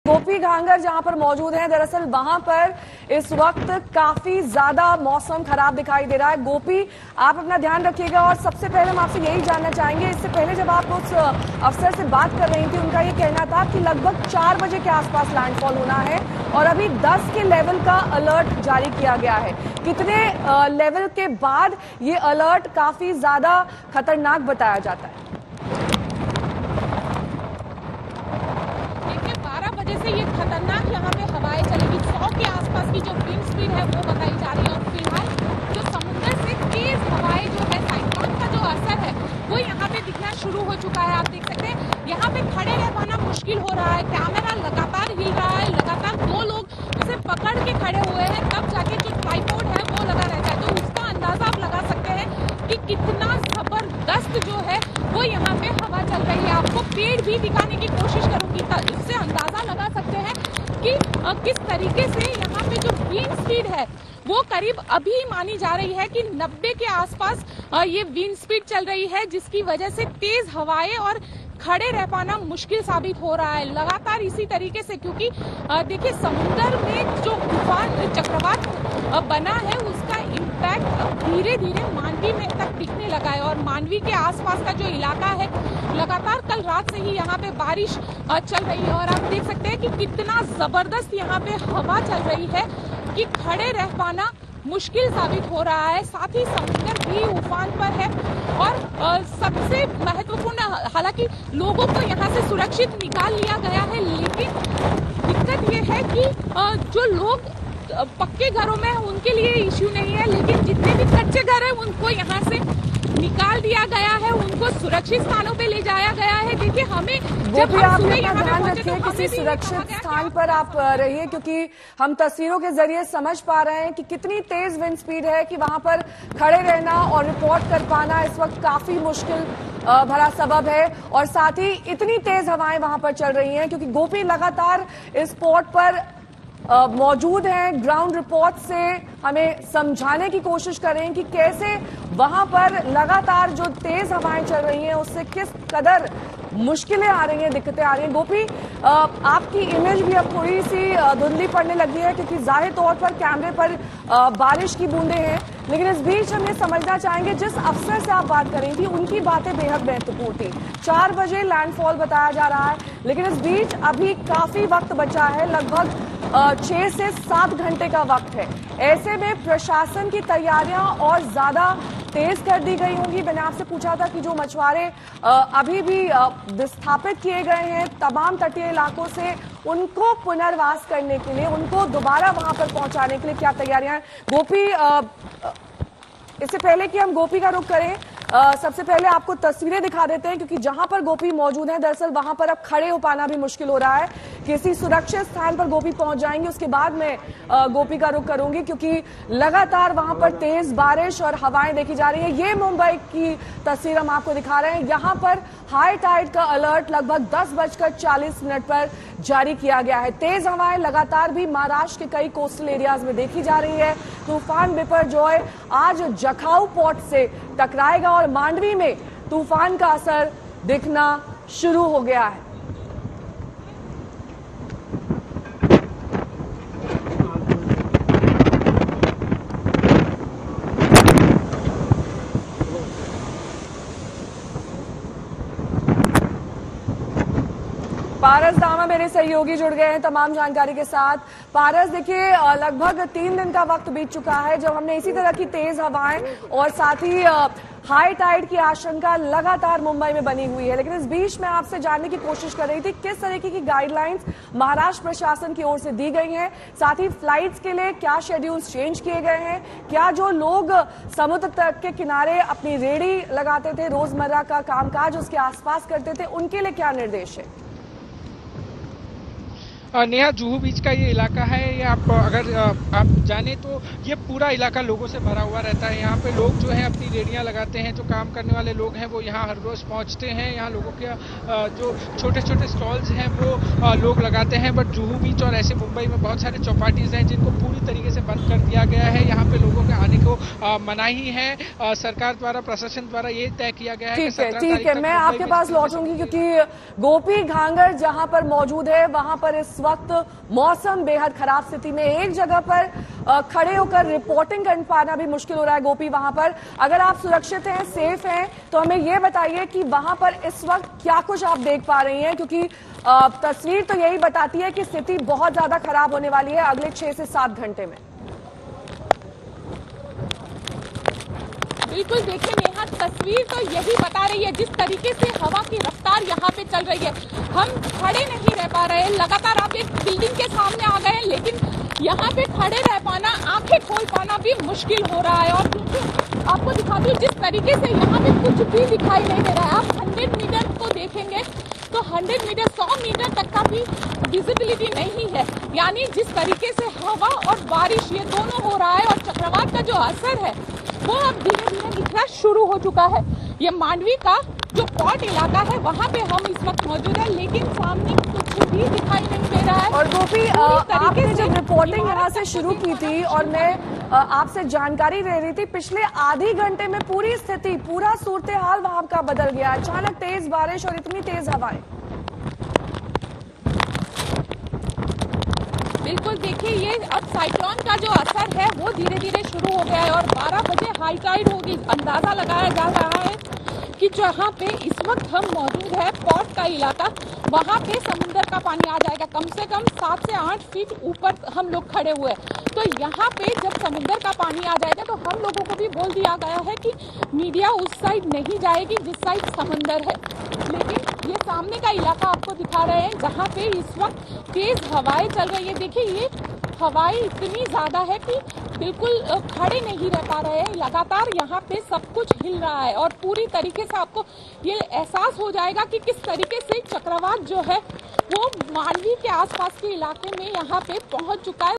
Cyclone Biparjoy Live Updates: Gujarat में लाइव रिपोर्टिंग के दौरान खतरनाक हुईं समंदर की लहरें,देखें
गुजरात पर मंडरा रहा है महातूफान बिपरडजॉय का खतरा तूफान पल-पसल करीब आता जा रहा है। आज शाम बिपरजॉय तूफान गुजरात के जखाऊ पोर्ट से टकराने वाला है. लेकिन इससे पहले ही मांडवी समेत गुजरात के कई इलाकों में बारिश शुरू हो चुकी है। समंदर में तेज लहरें उठ रही हैं। मौसम विभाग की मानें ये तूफान काफी शक्तिशाली है और इससे भारी विनाश मच सकता है। तूफान की वजह से कच्छ में समंदर में दो से तीन मीटर ऊंची लहरें उठ सकती हैं। पोरबंदर और द्वारका जिले में भयंकर मूसलाधार बारिश हो सकती है।